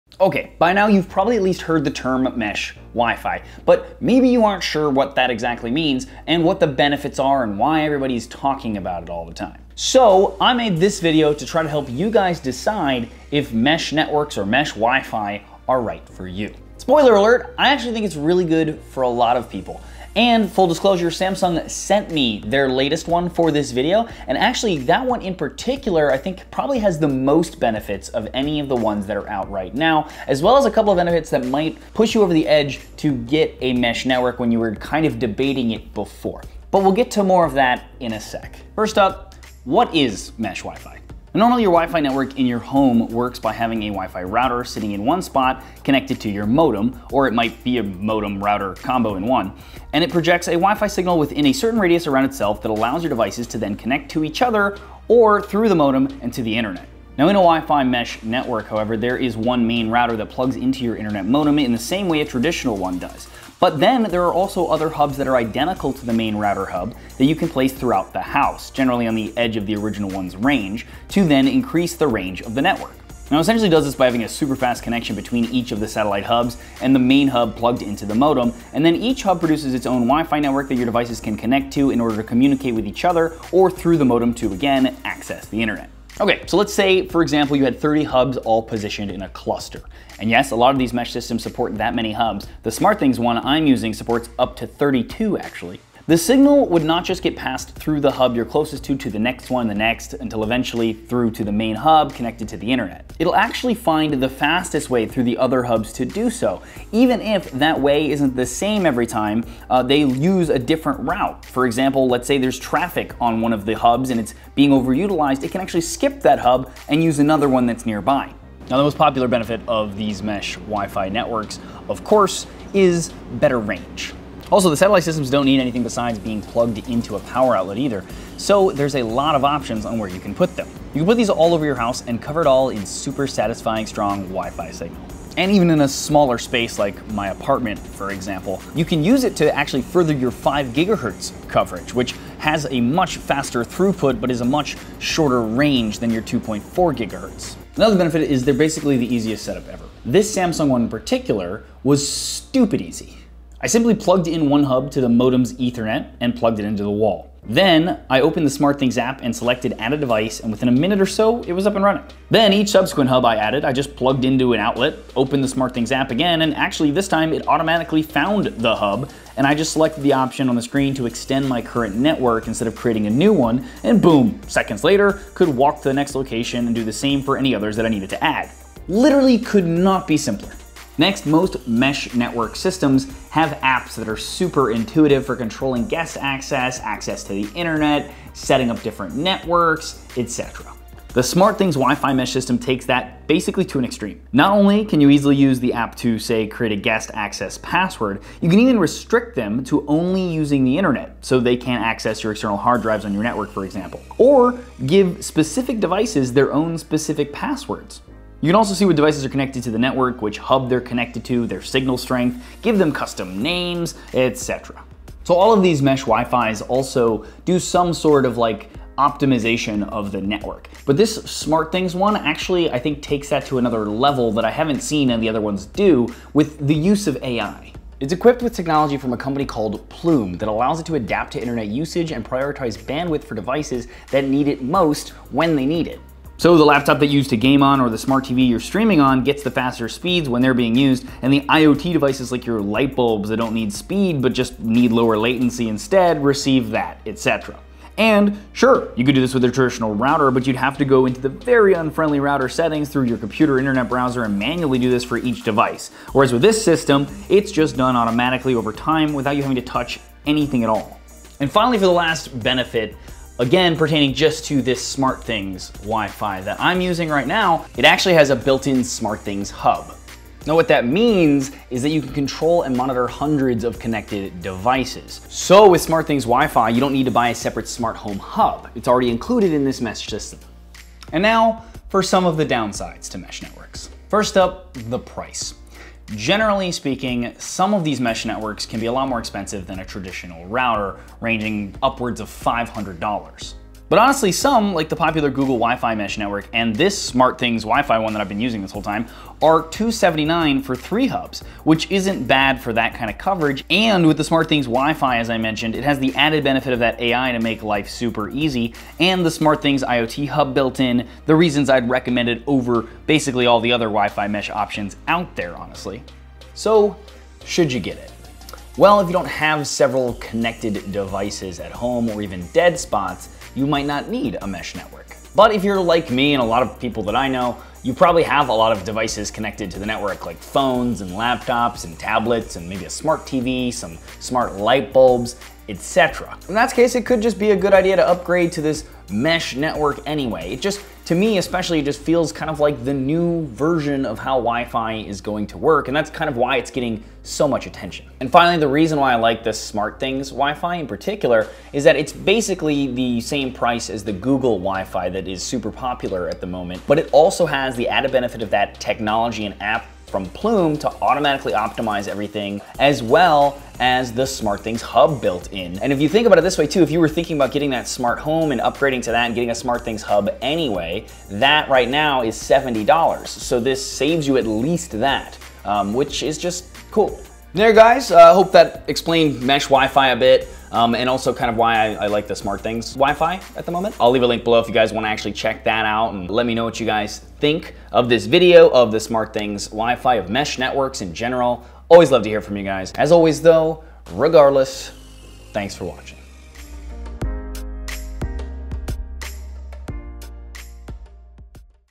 What is Mesh Wifi (& Why You Should Absolutely Get One)
OK, by now you've probably at least heard the term Mesh Wifi, but maybe you aren't quite sure what it is, the benefits of a it, etc. So I figured I'd break it down for you in this video so you can decide if a mesh network right for you. Spoiler alert: I think it's right for a lot of people and full disclosure Samsung sent me their newest one for this video, but there's a good reason why I think it is probably the most beneficial one out right now as well as the one with the most benefits.